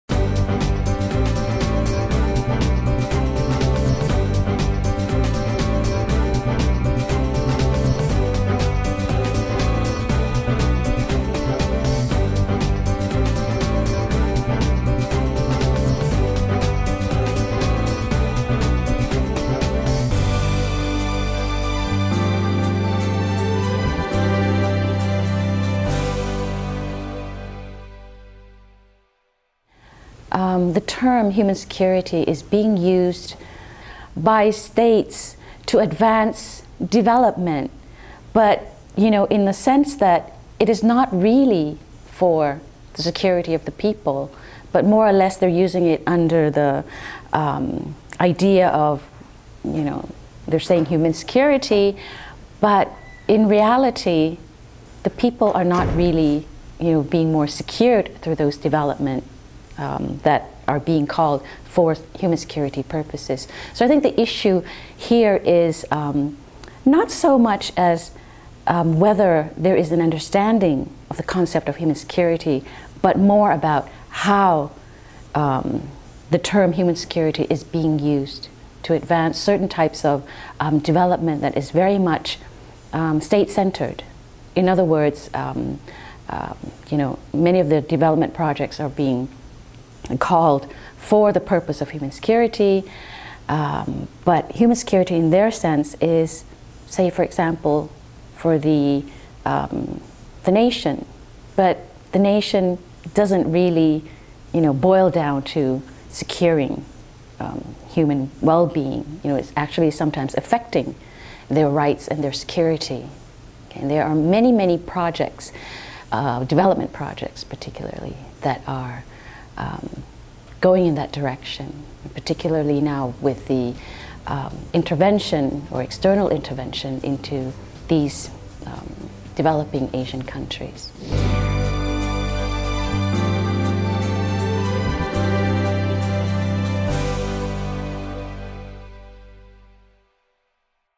Critical Issues Affecting Human Security in Asia: The Term 'Human Security'
Keokam Kraisoraphong, Assistant Professor at Chulalongkorn University, Bangkok, calls into question the way in which the term 'human security' is often used by nations yet this doesn't always boil-down to improvement in 'human wellbeing.'